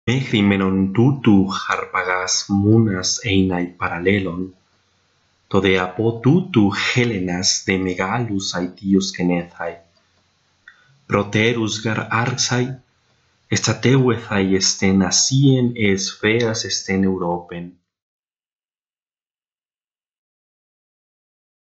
Ἡρόδοτος / Herodotus - 1.4.1
Haec lectio in schola graeca, quam magister Alexis Hellmer recitavit, lecta est.